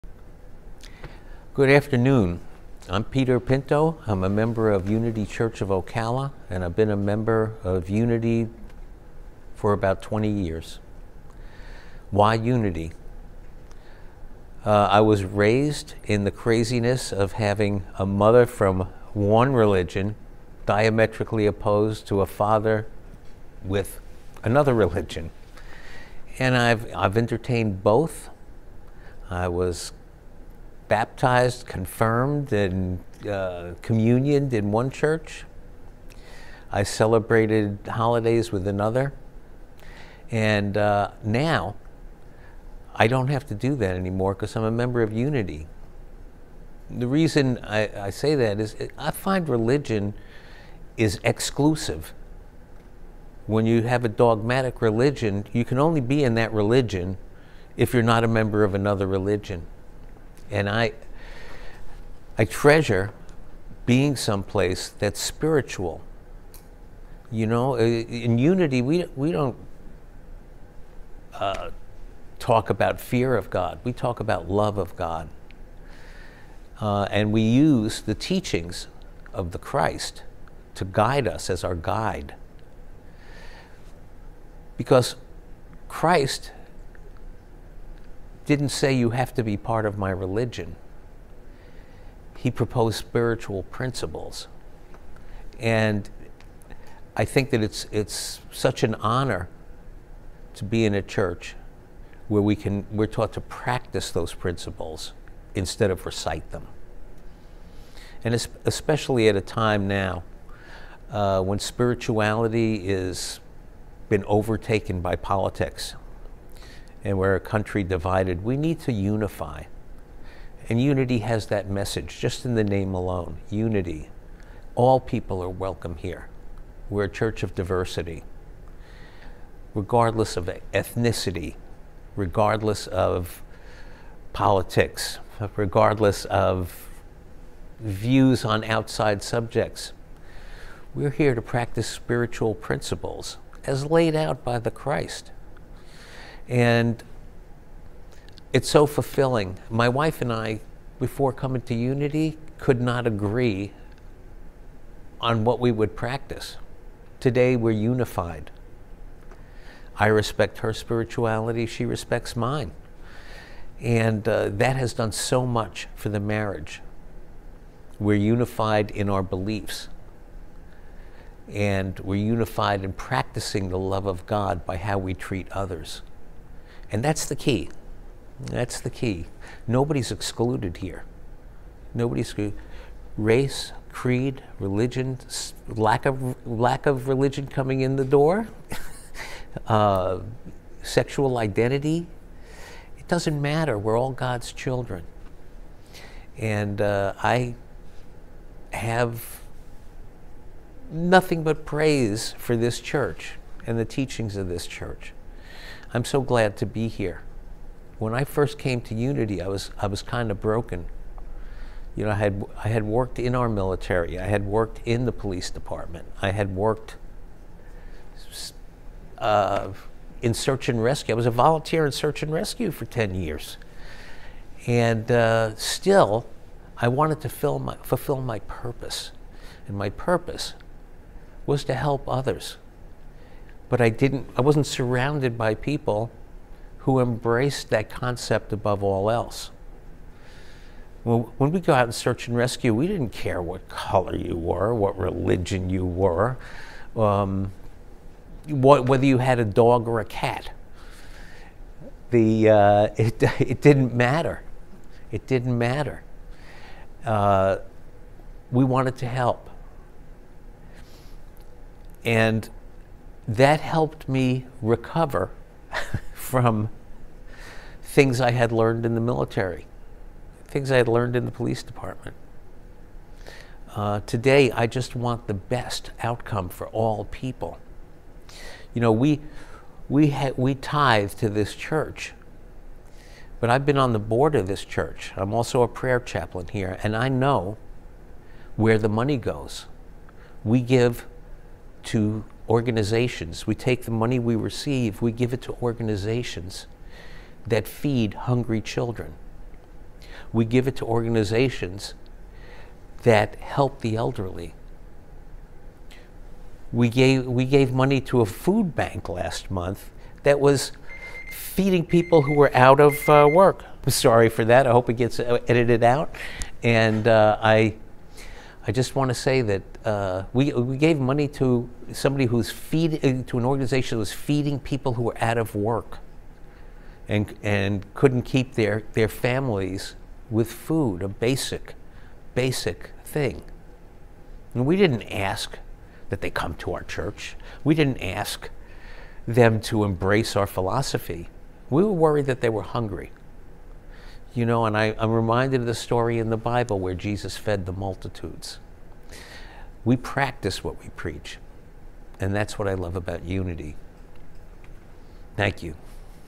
Inside Voices S1:E5 Peter Pinto
Peter stopped by today to share his thoughts on his spiritual journey and what brought him to Unity. Peter is someone whose life has been involved in both the military and the police and Unity has helped him to figure out how to deprogrammed those experiences with a spiritual practice that embraces love at it’s cornerstone.